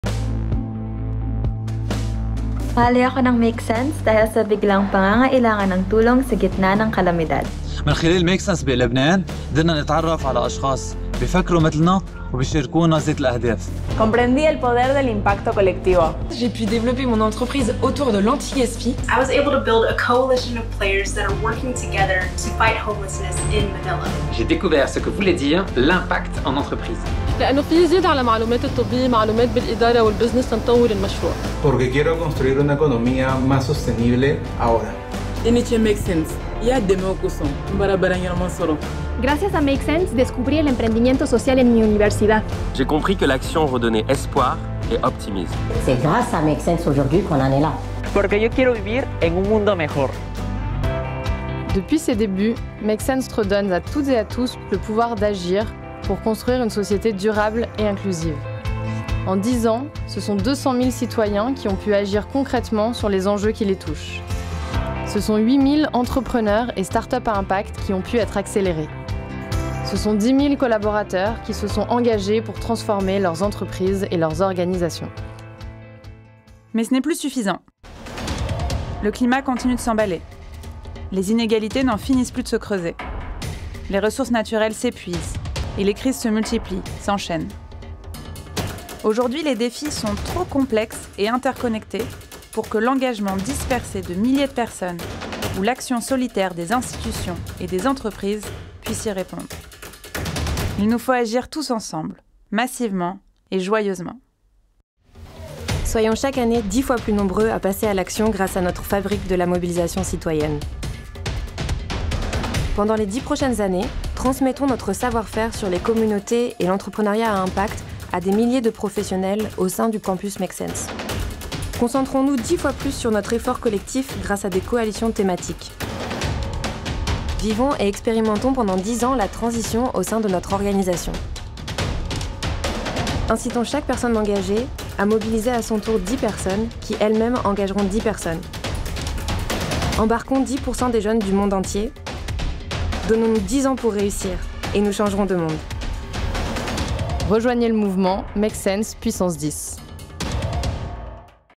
makesense Puissance 10
Prenez des bénévoles makesense du monde entier, faites le bilan des 10 dernières années, projetez-vous dans le futur… Et obtenez notre nouvelle vidéo : makesense puissance 10.